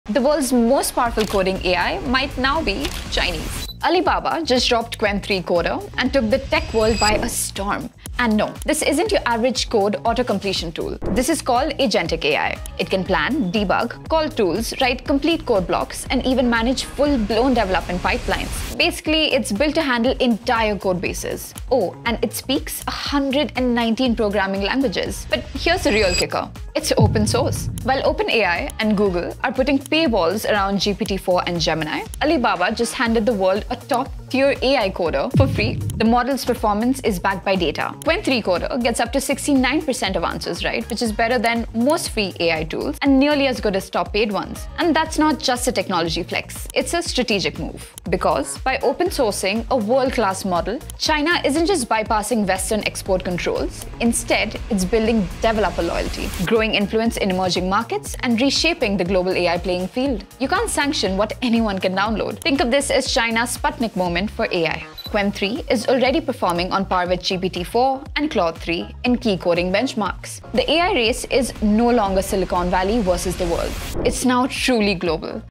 China launches GPT’s rival
Alibaba just launched Qwen3-Coder, a free AI that can code projects within seconds. With accuracy nearly close to GPT-4 and Claude 3, it’s one of the most powerful free AI tools ever released.
This isn’t just tech, it’s strategy. By open-sourcing Qwen3-Coder, China is winning over developers worldwide and shaking up the global AI race. AI is no longer a topic in trend, but a ground for innovations globally.